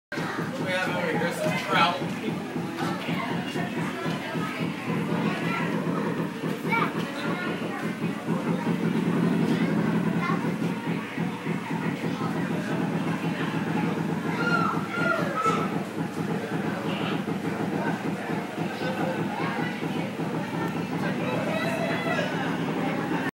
River or Pond Turtle
Ms. Brooks shares a video from the New England Aquarium.